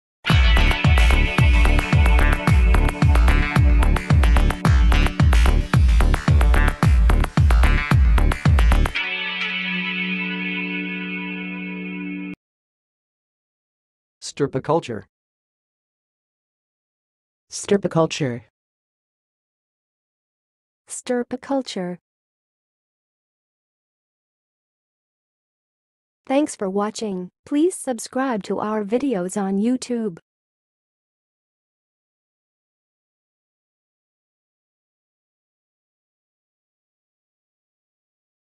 How to say "stirpiculture"! (High Quality Voices)
Watch in this video how to say and pronounce "stirpiculture"!